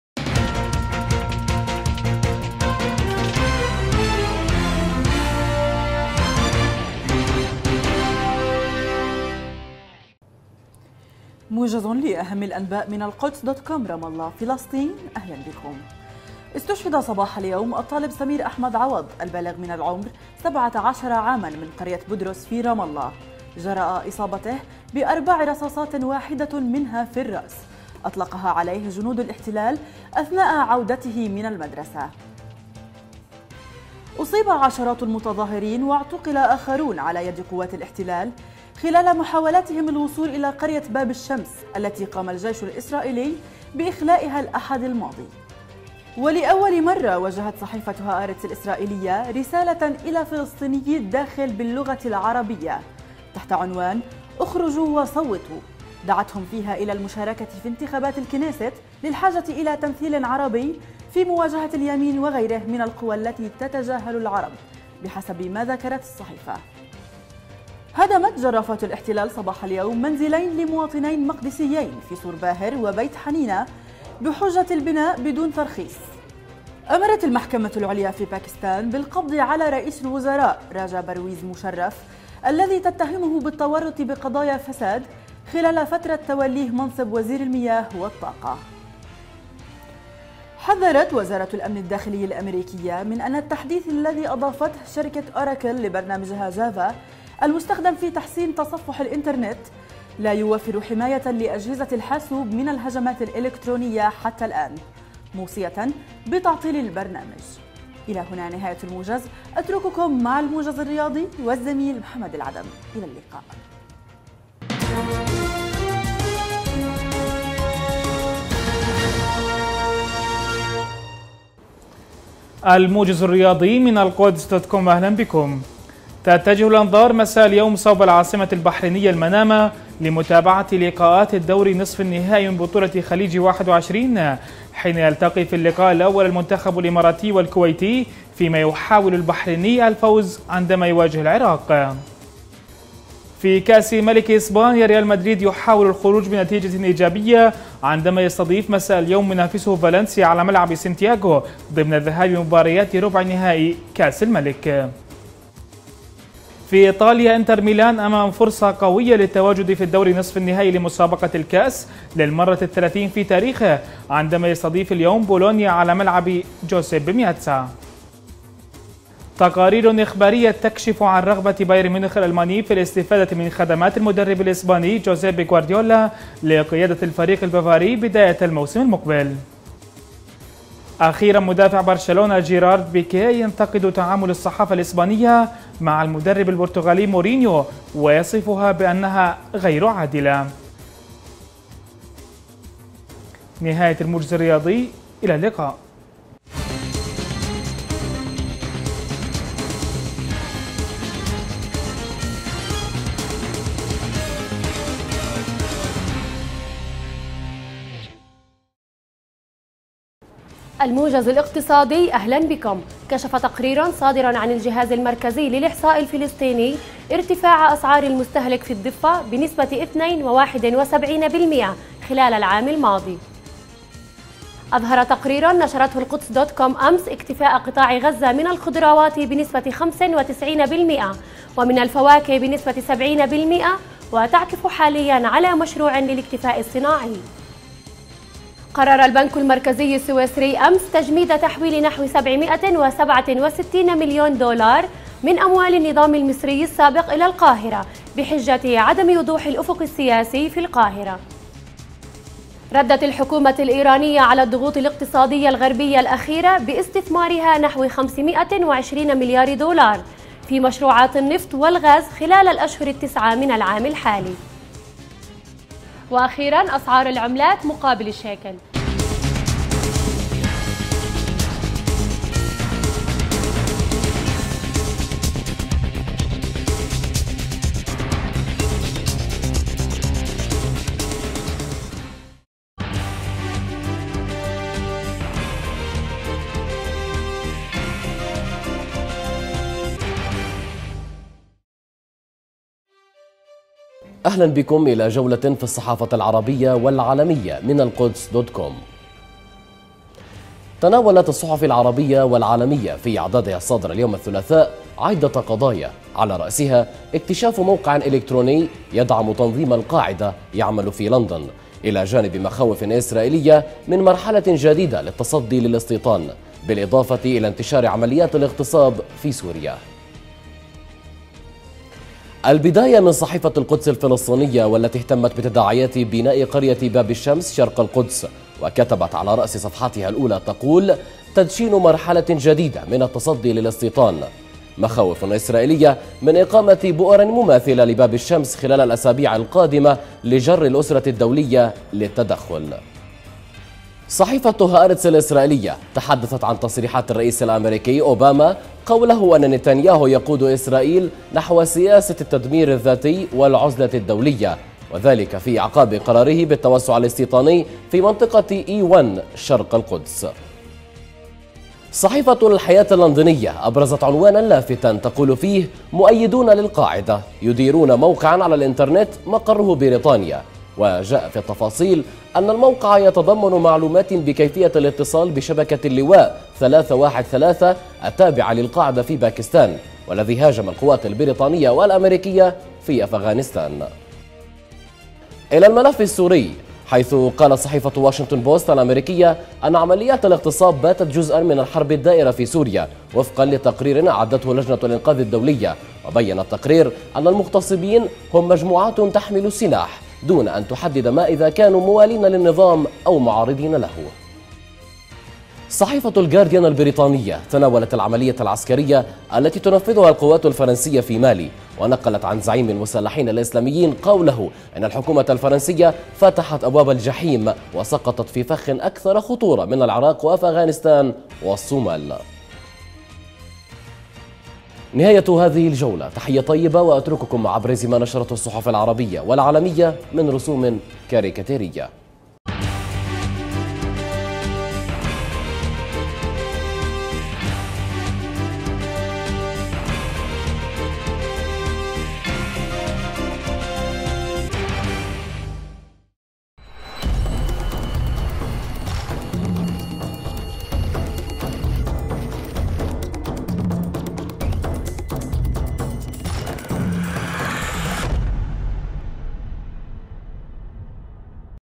استشهاد شاب بنيران الاحتلال في رام الله
هدم منزلين في القدس بحجة عدم الترخيص
أمر بالقبض على رئيس الوزراء الباكستاني لـ"الفساد"
 هآرتس تكتب مقالا بالعربية لأول مرّة
تحذير من أن "جافا" غير آمن إلى الآن